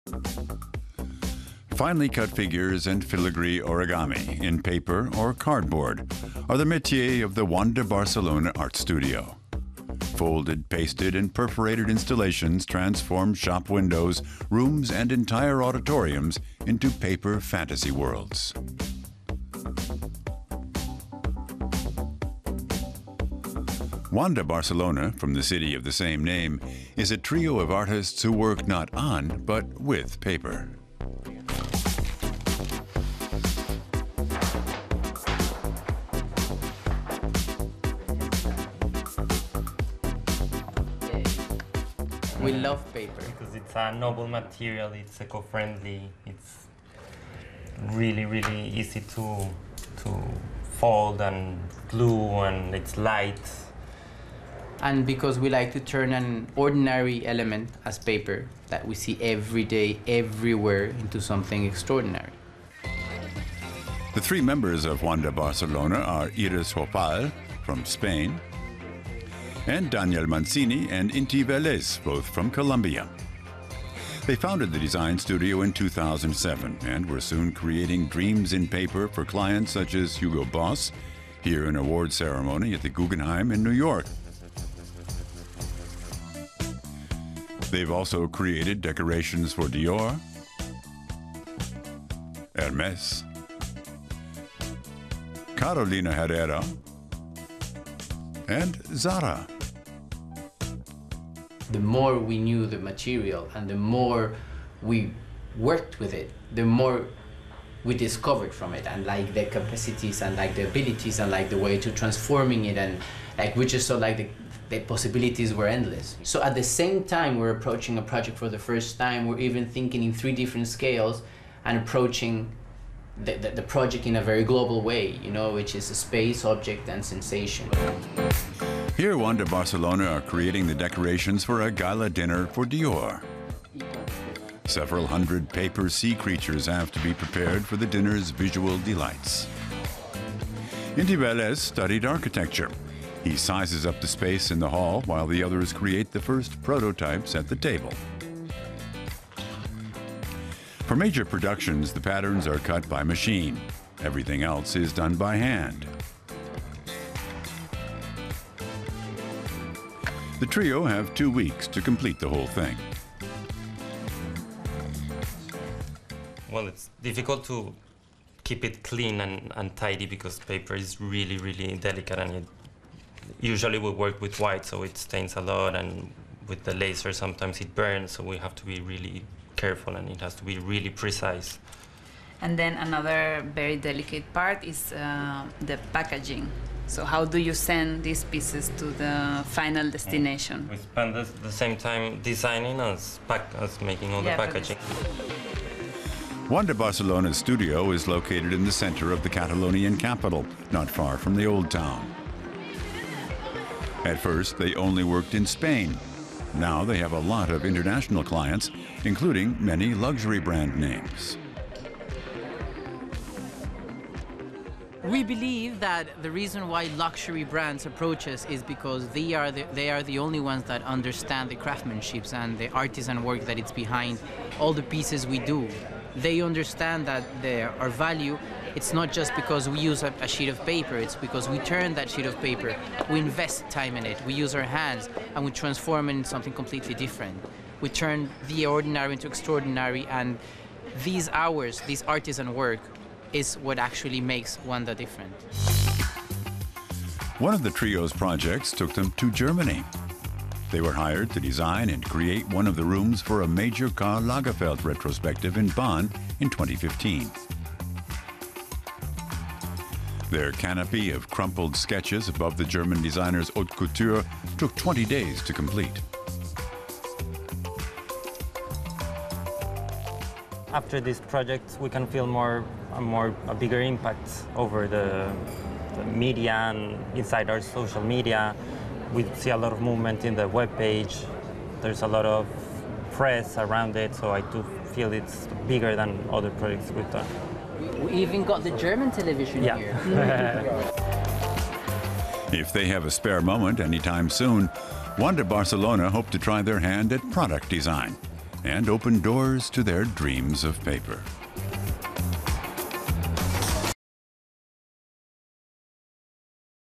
Paper art - Cut and folded | Euromaxx
Wanda Barcelona create delicate works of art from paper. The trio design phantasmagorical window displays and decorations for events, shows and luxury brand stores with the art of folded paper.